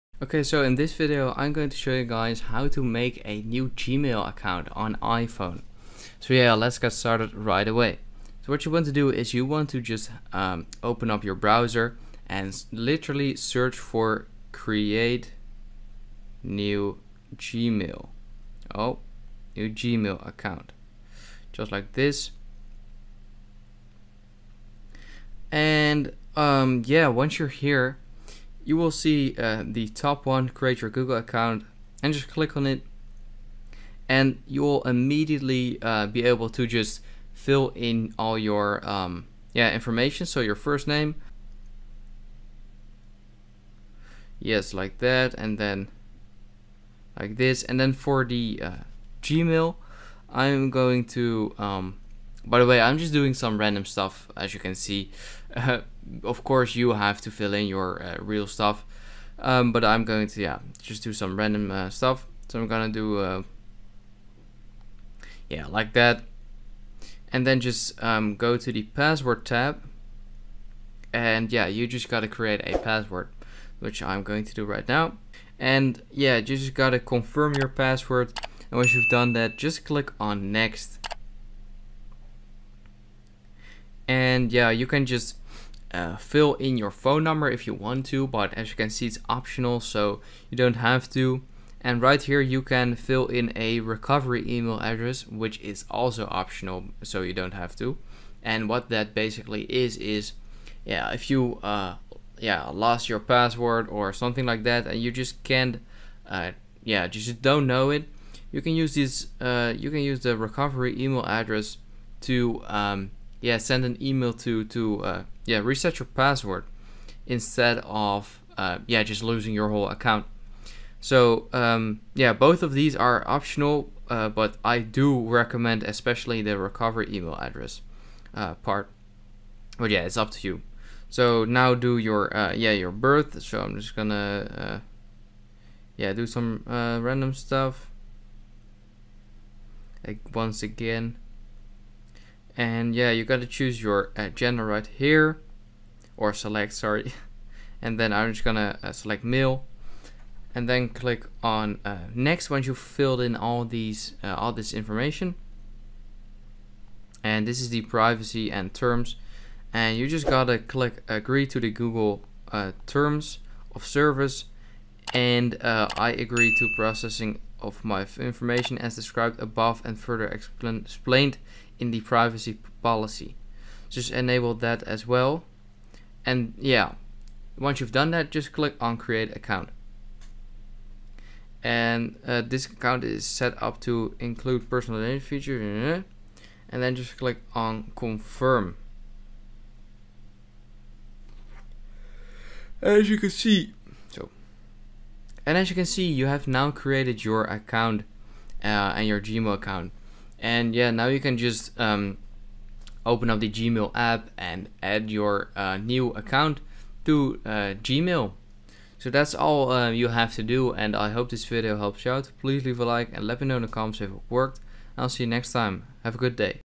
How to Make a New Gmail Account on iPhone! 2025
Learn How to Make a New Gmail Account on iPhone! 2025

In this video I show you how you can make a brand new gmail account on your iphone in 2025. This is a step by step tutorial that explains how to make new gmails to use.